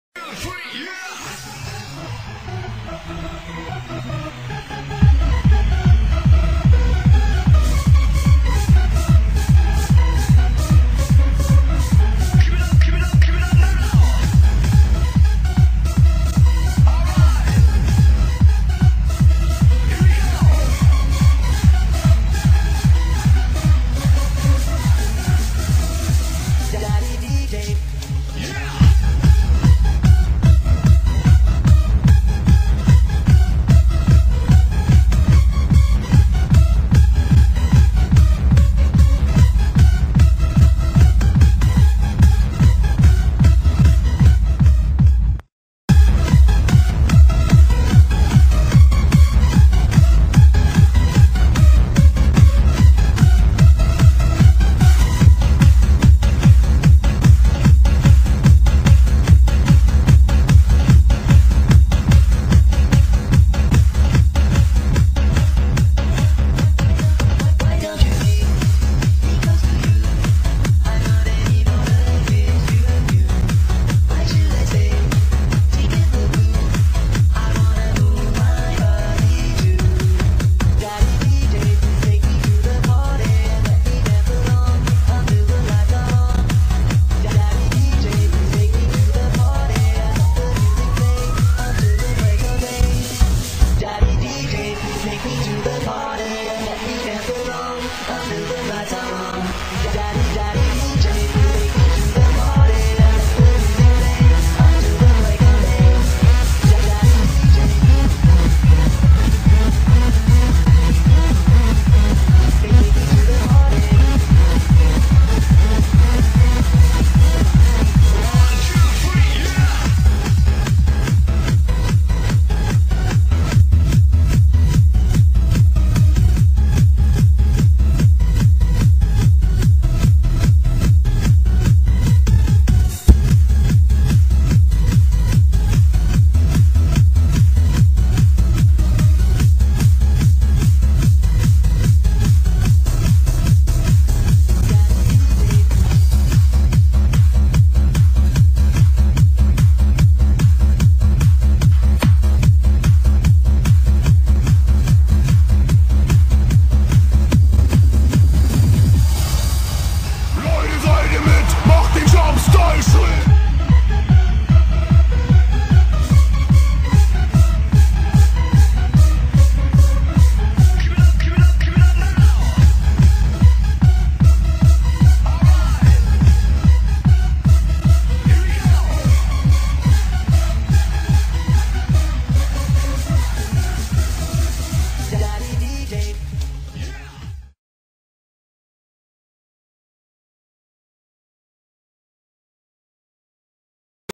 daddy dj
gibt mia ma tipps welches besser is dieses oda daddy dj 2 und wenns geht könntet ihr mir doch noxh so ein paar tips geben was man daran besser machen kann thx schonmal